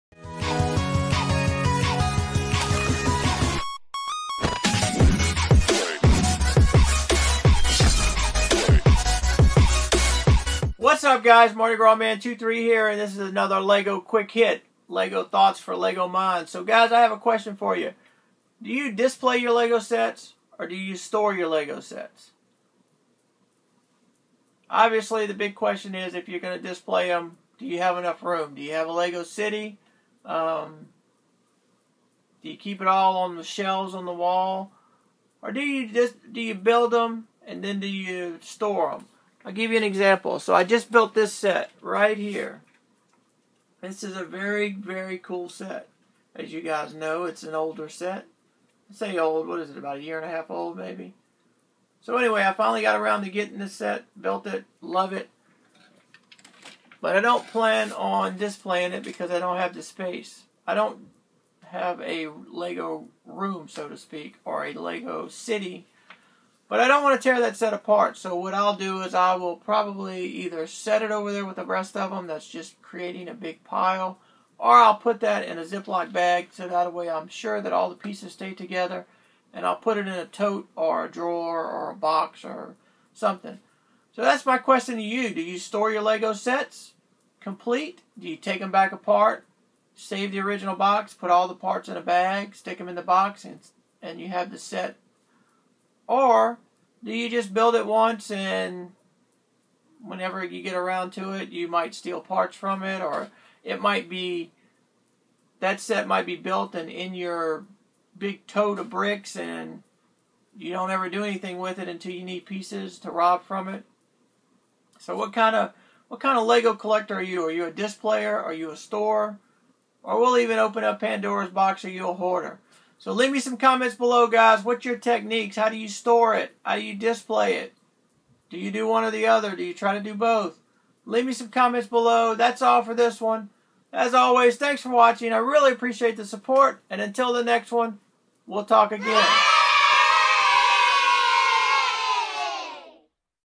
Lego Quick Hits #16 To Display Lego Set or To Store Lego Sets
Do you Store your sets, display your sets, or take your sets apart